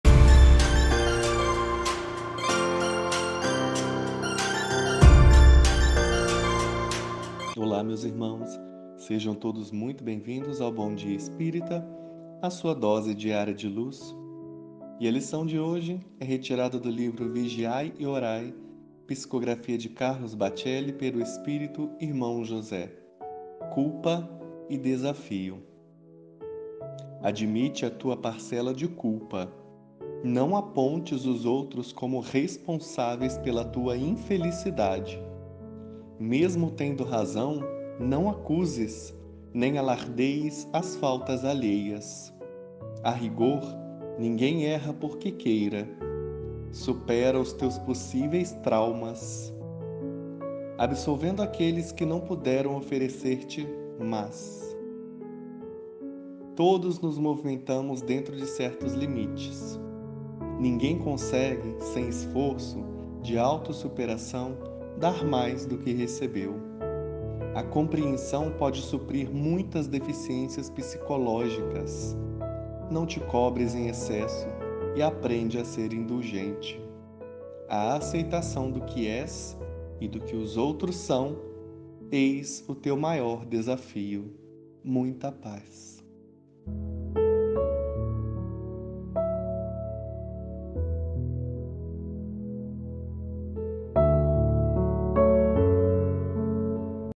Podcast 904 Bom dia Espírita, sua dose diária de luz!
Do livro Vigiai e orai, psicografado por Carlos Bacceli pelo espírito irmão José   Lição: “Culpa e desafio”
Centro Cristão Espírita Zico Horta : Av. Presidente Kennedy,925 Ano Bom – Barra Mansa RJ